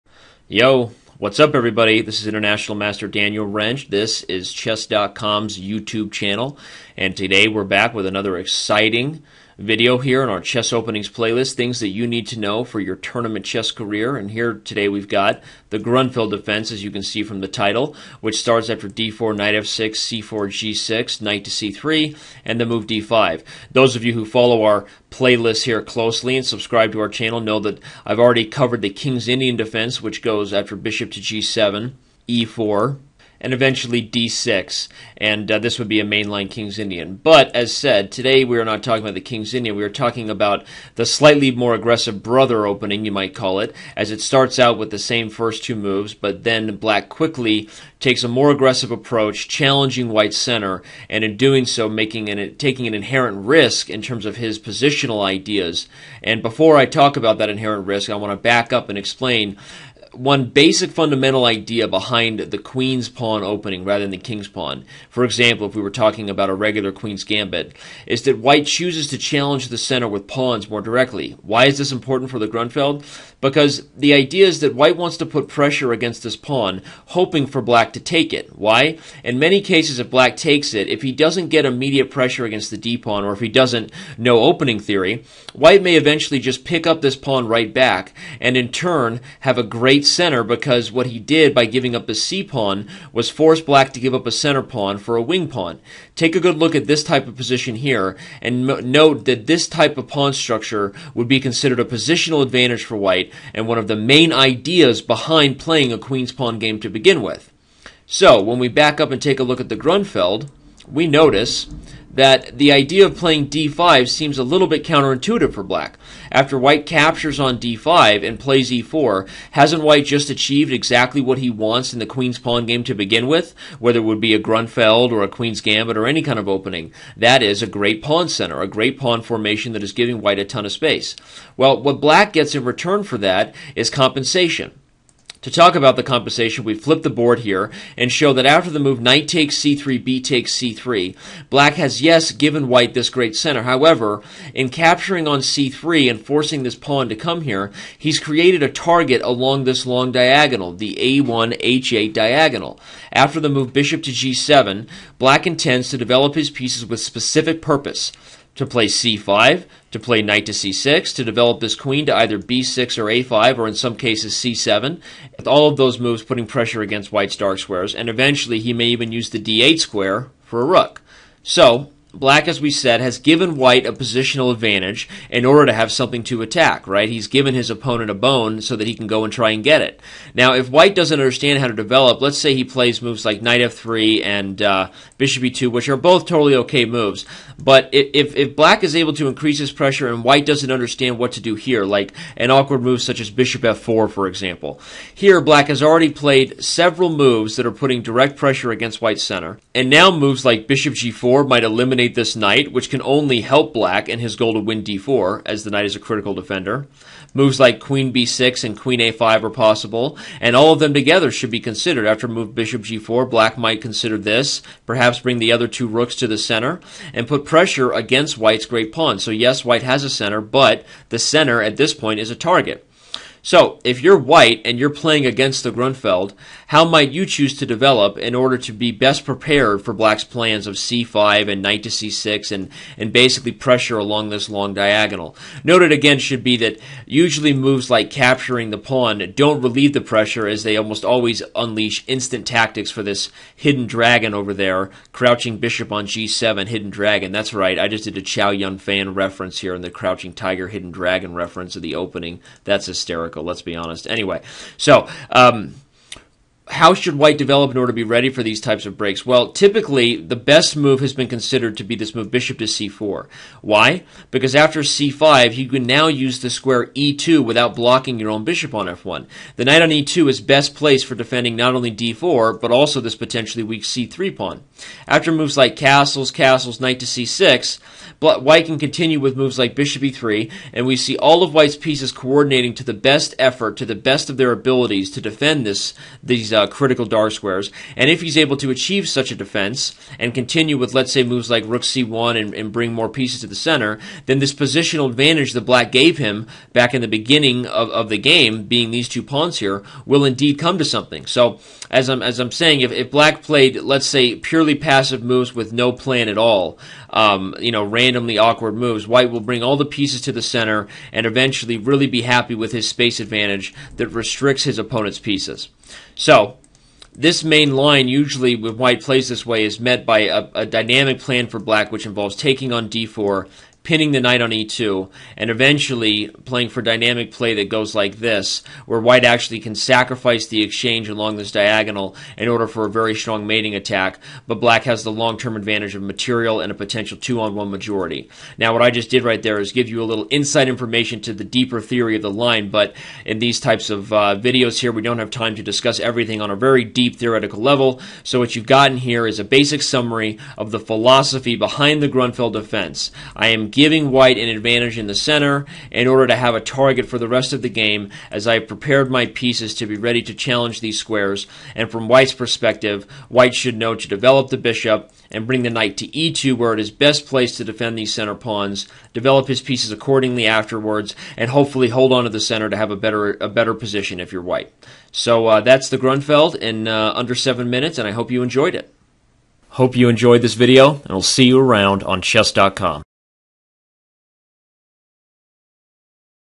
Chess Openings: How to Play the Grunfeld Defense!
Another great video on Chess.com's YouTube channel, another main line opening reviewed in fast paced fashion! For your viewing pleasure, IM Daniel Rensch breaks down the ideas behind the Grunfeld on a fundamental level that should also better your understanding of the purpose behind 1.d4!